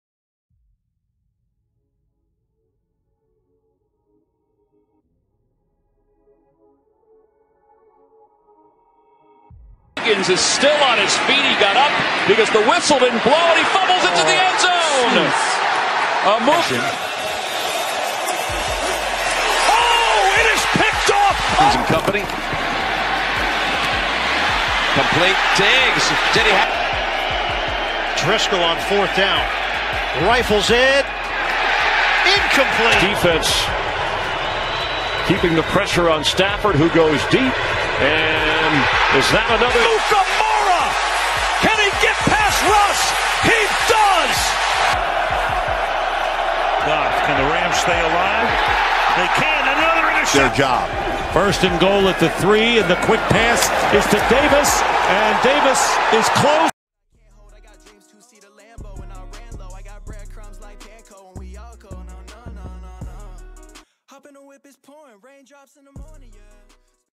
NEW ORLEANS SAINTS: Prince Amukamara ᴴᴰ
BREAKING NEWS || NEW ORLEANS SAINTS || PRINCE AMUKAMARA HIGHLIGHTS
113 CAREER GAMES
476 TACKLES
10 INTERCEPTIONS
6 FORCED FUMBLES